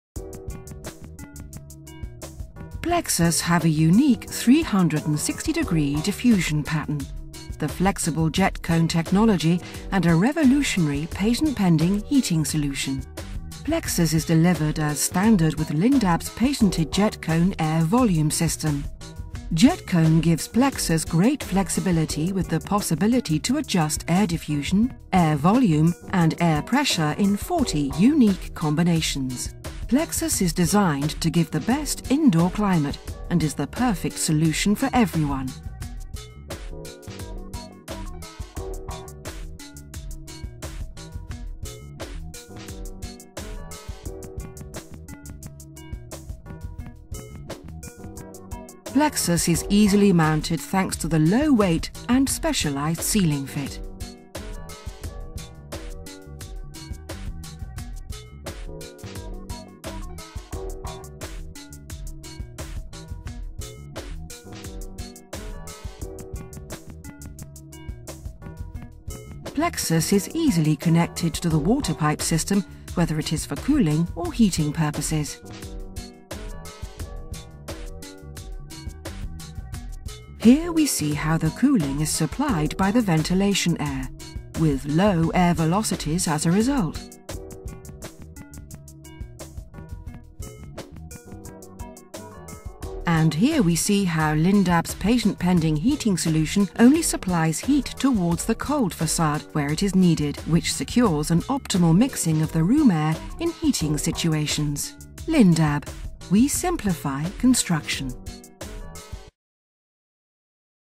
Plexus from Lindab - We simplify construction...
Plexus represents a world class solution for the ideal indoor climate. It is an all-in-one product that takes care of the room's ventilation, heating and cooling needs in a discreetly powerful way. The design relies on two unique techniques -- 360° air distribution and the new patent-pending JetCone technology. This means no draught, no noise and no problems -- before, during and after the HVAC installation. With Plexus, you can equip the building with a solution that offers new possibilities for an optimized room comfort with lower energy consumption, a simplified installation and a problem-free future when room layout adaptations are needed or reconstructions are required. Plexus eliminates the need for a radiator system to be installed in each room and it brings a new level of economic and architectural advantages.
Enter a world of freedom, security and economy and let Plexus take care of your indoor climate.